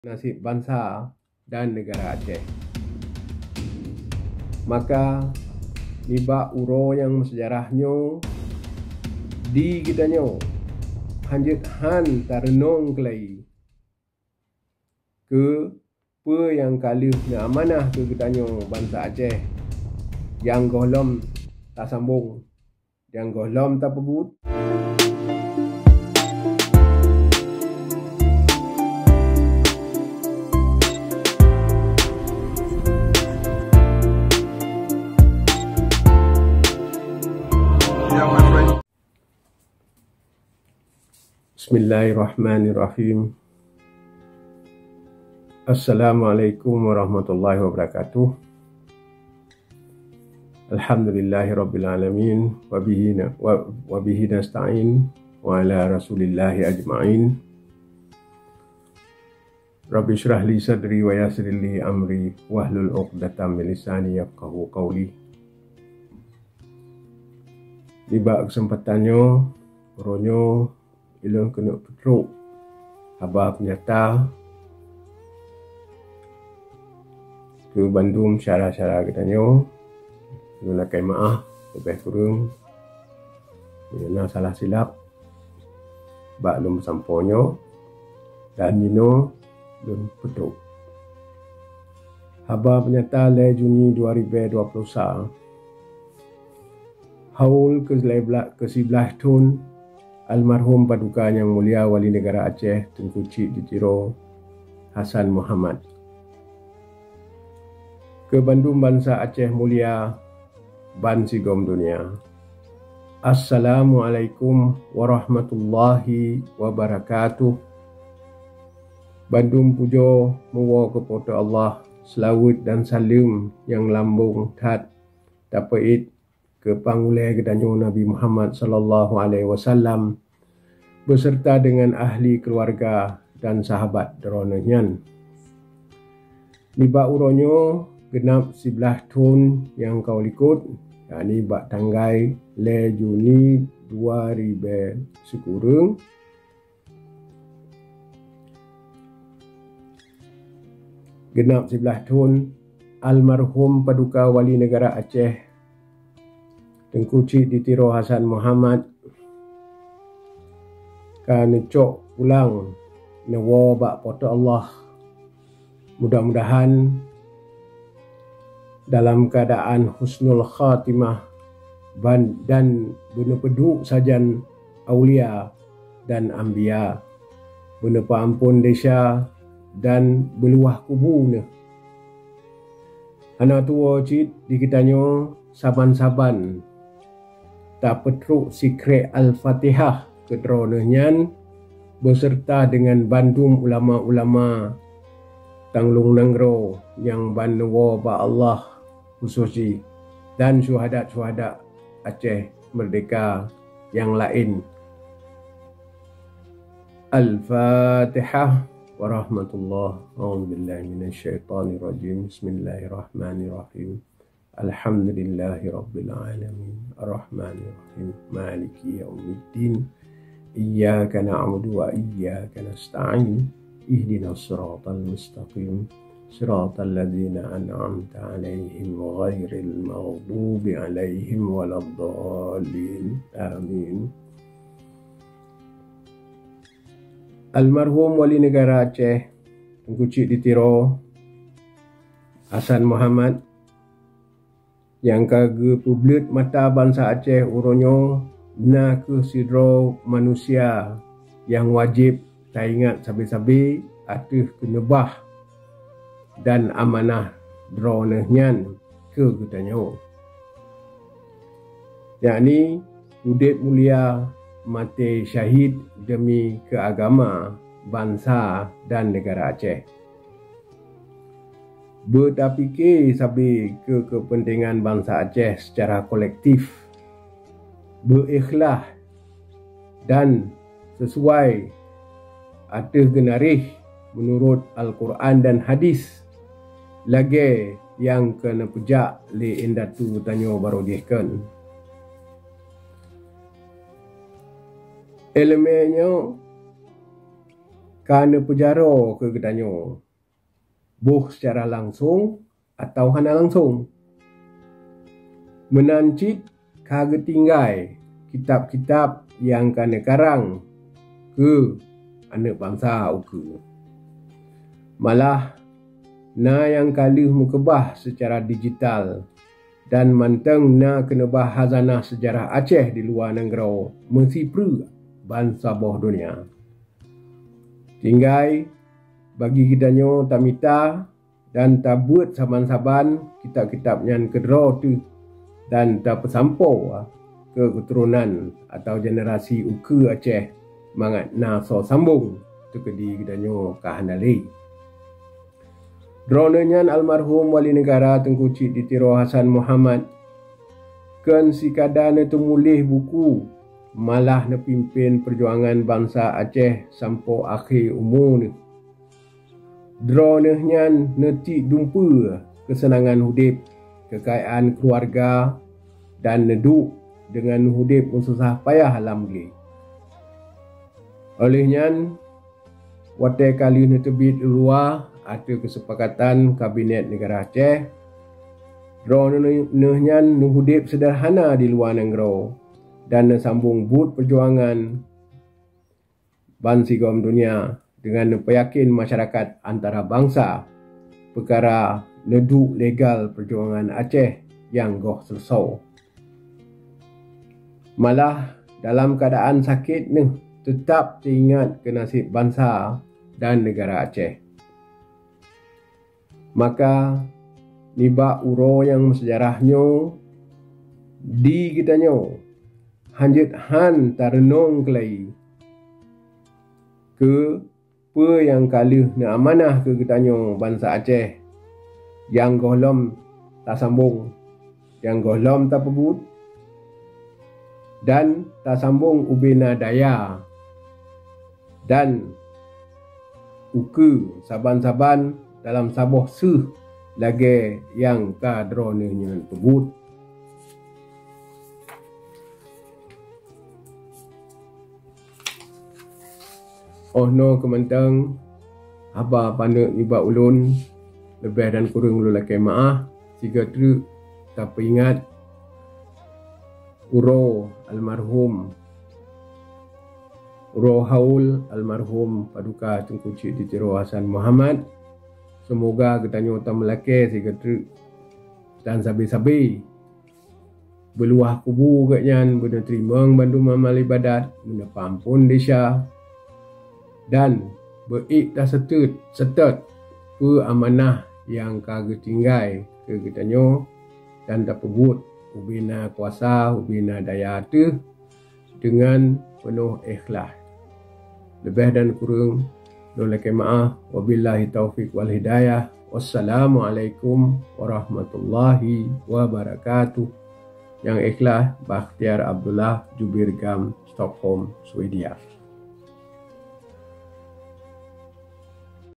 HAUL keu 11 Thôn Almarhum Paduka Njang Mulia, Wali Neugara Atjèh,Tengku Tjhik di Tiro Hasan Muhammad
Keu bandum bansa Atjèh mulia ban sigom dônja,
Bandum pudjoë meuwoe keu Po teuh Allah. Seulaweuët dan saleuëm njang lambông ta peu-ét keu Pang Ulèë geutanjoë, Nabi Muhammad Sallallahu ’alaihi wasallam beuseurta deungon ahli keuluarga dan sahabat2 droëneuhnjan.

Nibak uroënjoë, geunap 11 thôn Almarhum Paduka Wali Neugara Atjèh neutjôk pulang-neuwoë bak Po teuh Allah. Mudah2an dalam keuadaan husnul khatimah dan beuneupeuduëk sadjan aulia dan anbija.Beuneupeu-ampôn dèësja dan beuluwaih kubu. Hana tuwô tjit di geutanjoë saban2 ta peutrôk sikrèk Al fatihah keu droëneuhnjan beuseurta deungon bandum ulama2 njang ban neuwoë bak Allah chususdjih dan sjuhada2 njang laén. Al fatihah.
Almarhum Wali Neugara, njang ka geupeubleuët mata bansa Atjèh uroë, nakeuh sidroë manusia njang wadjèb ta ingat sabé2 ateuëh keuneubah dan amanah droëneuhnjan keu geutanjoë. Njakni hudép mulia – maté sjahid deumi keu agama, bansa dan dan Neugara Atjèh. Piké keu keupeunténgan bansa Atjèh sitjara kollektif, beu-échlaih dan lagèë meunurôt Al Quran dan Hadith, lagèë njang ka neupeudjak lé éndatu geutanjoë baroëdjéhkôn.

Èleumèë njan ka neupeudjaroë keu geutanjoë boh sitjara langsông atawa hana langsông.  Meunantjit ka geutinggai kitab2 njang ka neukarang keu aneuk bansa. Malah na njang ka lheuëh meukeubah sitjara digital dan mantông na keuneubah hazanah seudjarah Atjèh diluwa nanggroë, meusipreuëk ban saboh dônja. Tinggai ta mita dan ta beuët kitab2njan keudroë-teuh dan teupeusampoë keu keuturônan (generasi) ukeuë mangat na soë sambông.

Droëneuhnjan, Alm. Wali Neugara, Tengku Tjhik di Tiro Hasan Muhammad, kôn sikada neuteumuléh buku malah neupimpin peurdjuangan bansa Atjèh sampoë akhé umu-neuh. Droëneuhnjan neutiëk dumpeuë keuseunangan hudép – keukajaan, keuluarga dan neuduëk, deungon neuhudép meusôsah-pajah lam glé. Lheuëhnjan neuhudép seudeurhana di luwananggroë dan neusambông buët peurdjuangan deungon neupeujakin masjarakat antarabansa peukara neuduëk legal peurdjuangan Atjèh njang goh seuleusoë. Malah dalam keuadaan sakét-neuh teutap teu-ingat keu nasib bansa dan neugara Atjèh.

Maka nibak uroë njang meuseudjarahnjoë di geutanjoë handjeuët han ta reunông keulai, keu peuë njang ka lheuëh neuamanah keu geutanjoë bansa Atjèh dan ta sambông ubèna daja ukeuë saban2 dalam saboh seuêh lagèë njang droëneuhnjan peubuët.

Wabillahi taufiq walhidajah wassalamu alaikum warahmatullaahiwabarakaatuh.

Njang échlaih,
Bakhtiar Abdullah
Djubir GAM
Stockholm, Swedia.